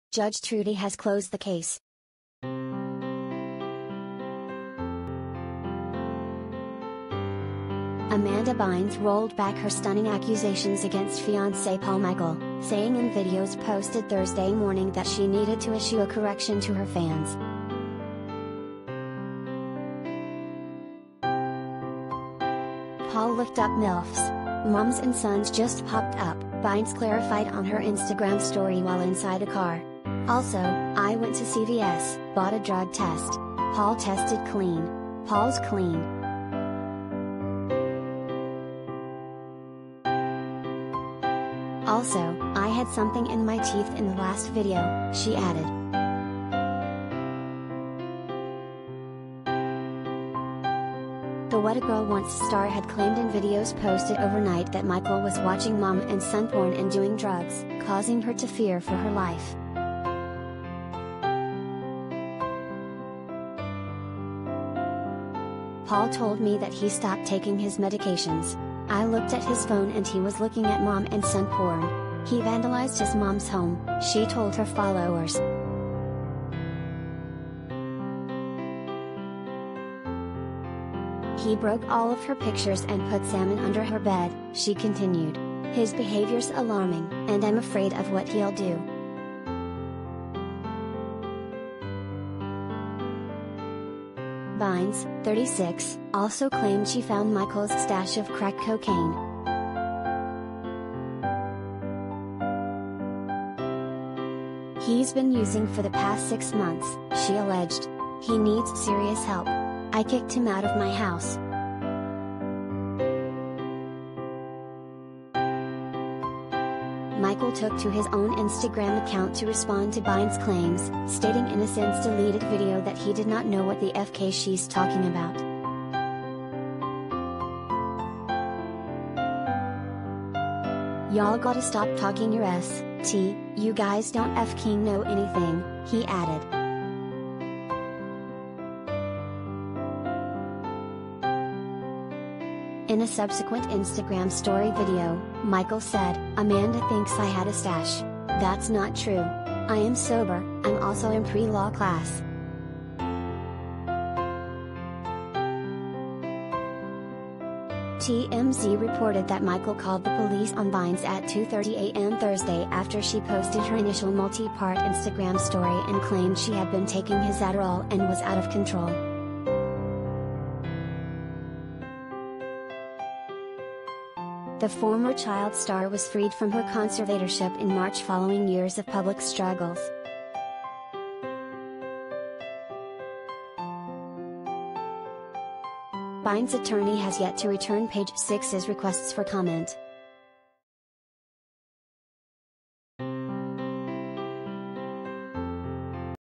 Amanda Bynes clarifies fiancé watched ‘MILF’ porn, not ‘mom-and-son’
Judge Trudy has closed the case.
Amanda Bynes rolled back her stunning accusations against fiancé Paul Michael, saying in videos posted Thursday morning that she needed to issue a “correction” to her fans.
“Paul looked up MILFs. Moms and sons just popped up,” Bynes clarified on her Instagram Story while inside a car. “Also, I went to CVS, bought a drug test. Paul tested clean. Paul’s clean.
“Also, I had something in my teeth in the last video,” she added.
The “What a Girl Wants” star had